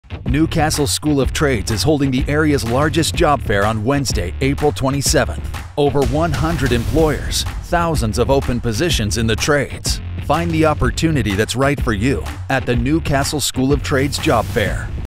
NCST 2022 Job Fair 15 sec GOLD TV Spot
Fifteen-second television spot showcasing the Gold sponsors for the New Castle School of Trades 2022 Job Fair on April 27.